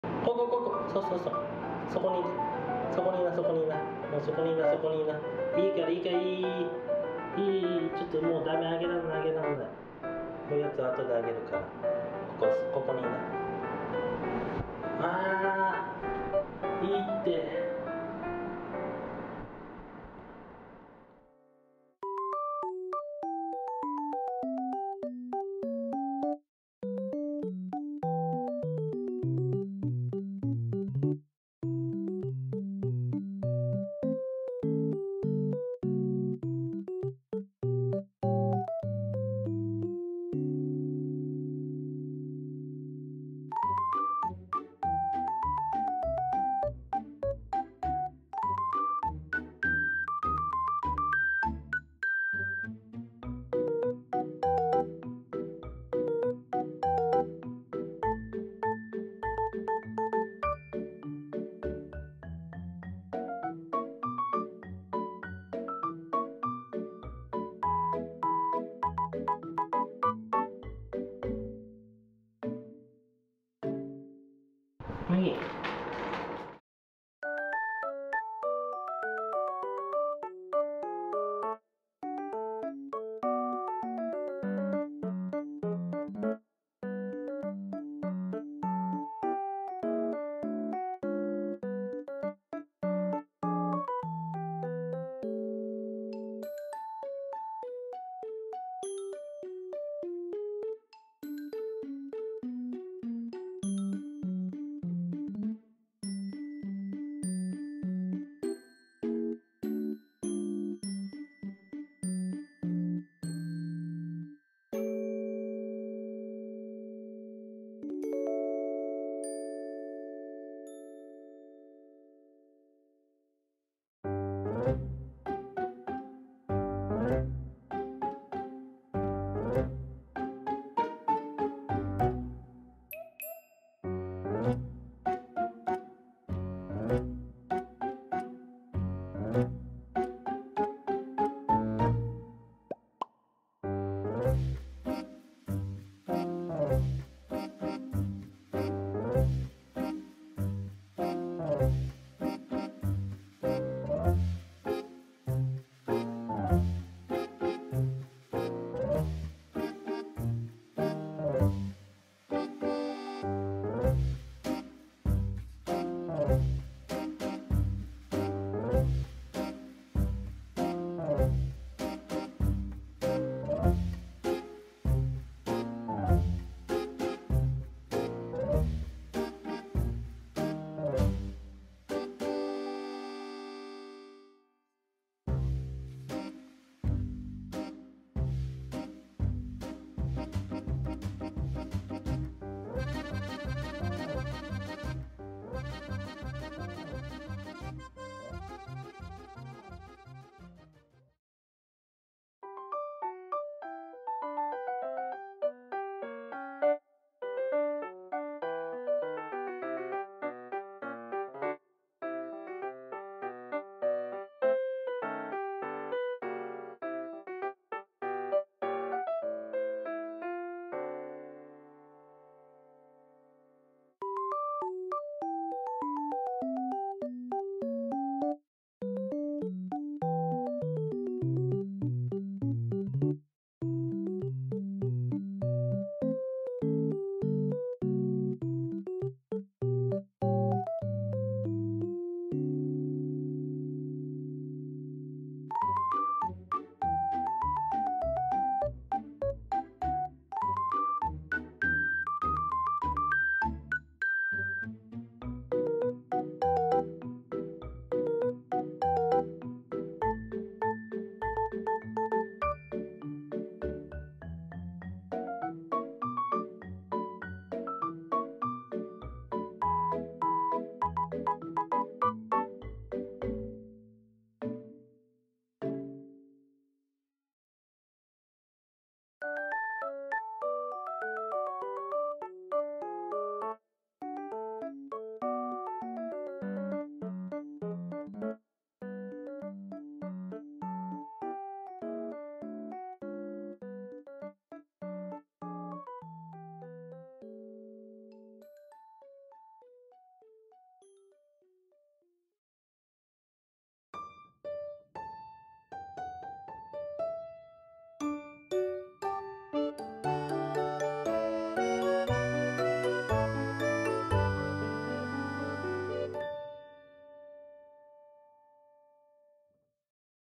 高い位置から落下して左前足を痛めてしまったので急遽病院へ行ってきました【No.834】
Netherlands Dwarf  (Mugi)🐰

使用BGM